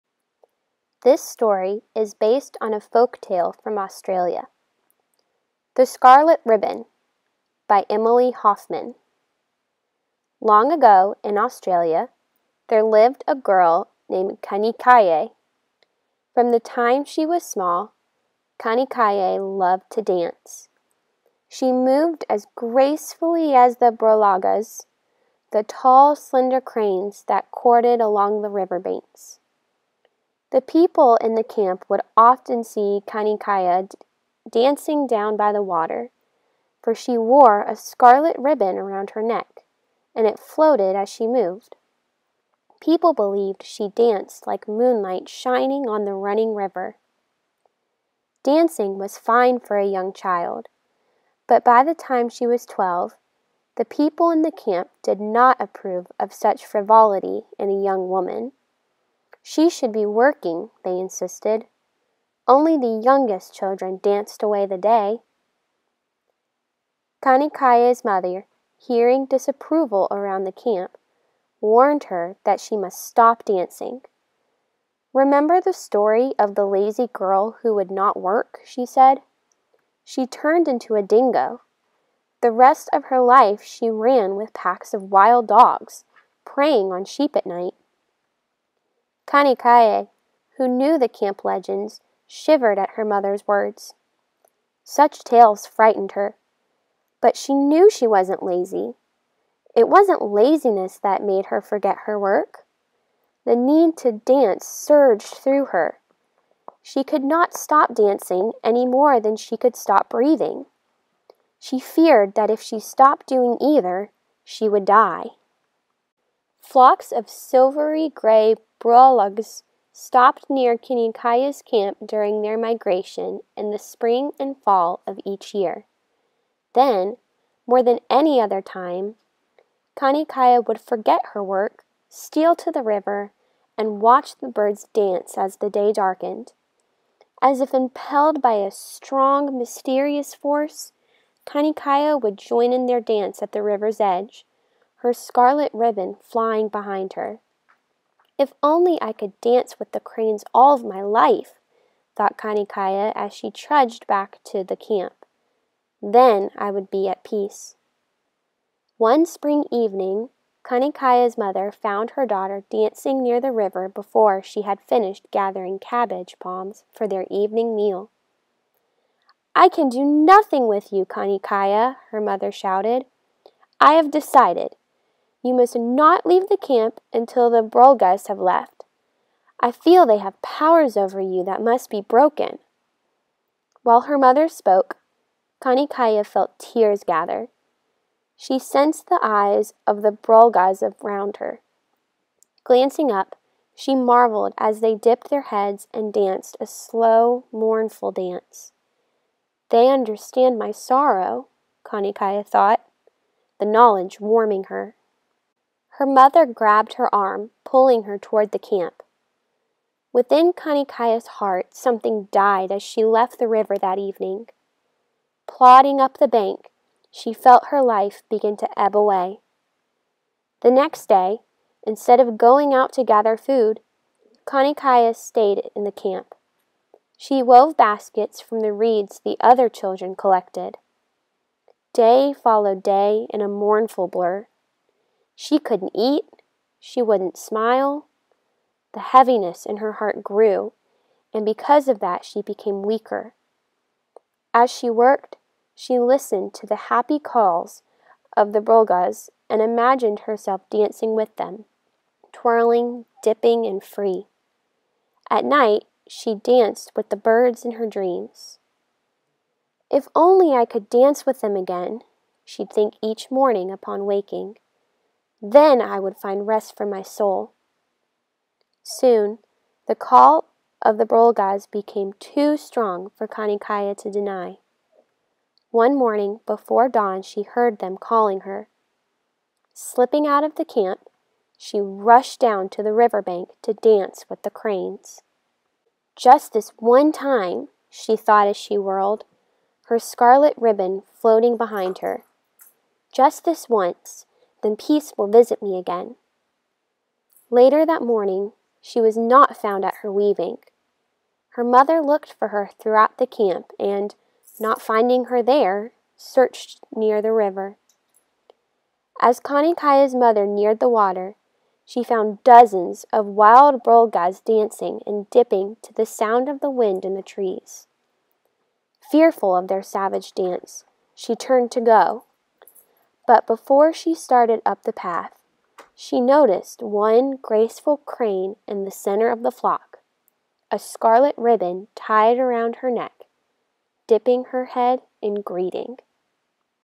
The Scarlet Ribbon
This video is about The Scarlet Ribbon. It is a read aloud created for distance learning during COVID-19.